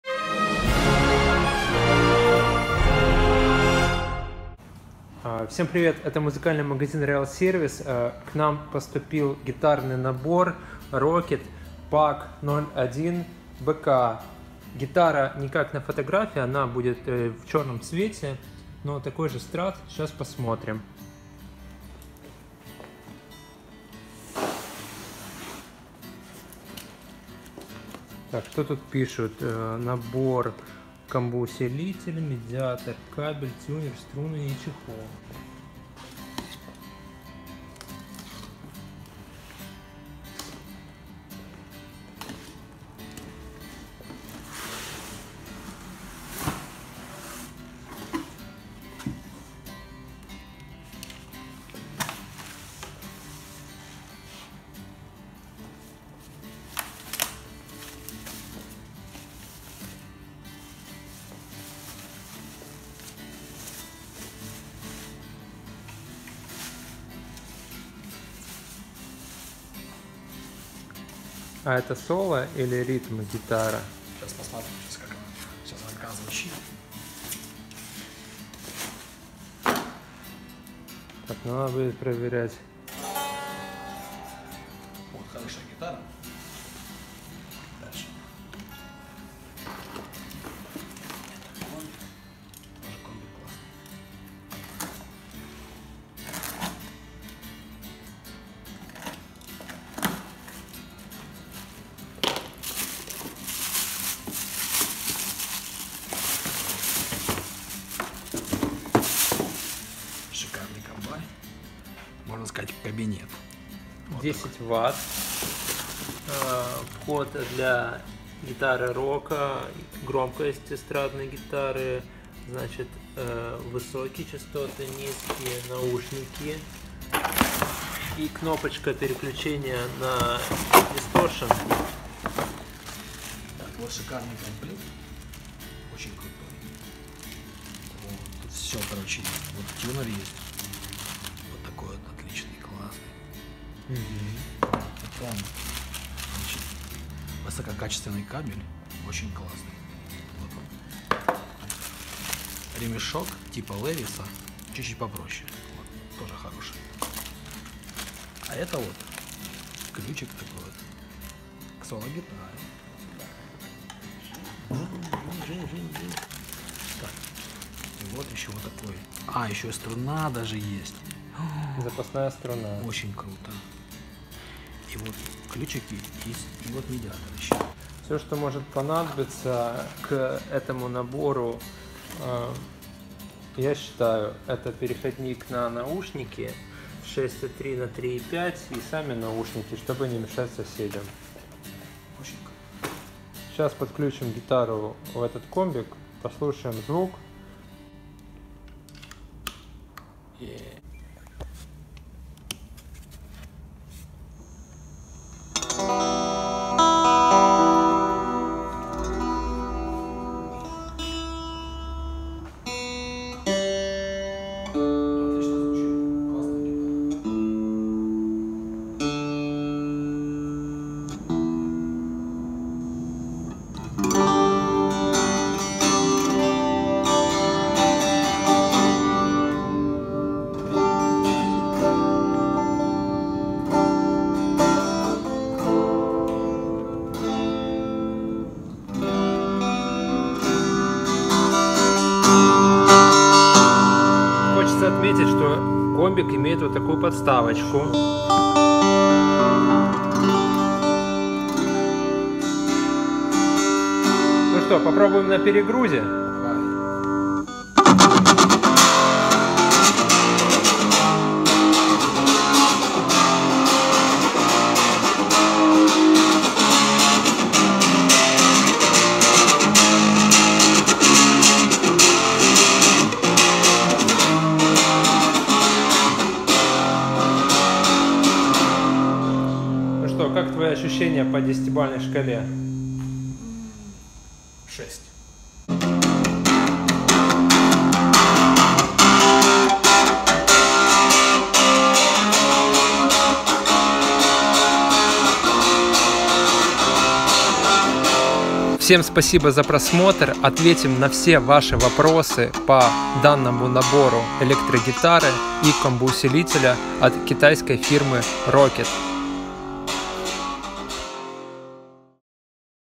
Электрогитарный набор ROCKET PACK-1 (обзор)
К нам поступил гитарный набор ROCKET PACK-1, сделали так называемый анбоксинг, настроили струны буквально один раз, подключили в комбоусилитель, который идёт в наборе.

Гарантия на комбик и электрогитару - 12 месяцев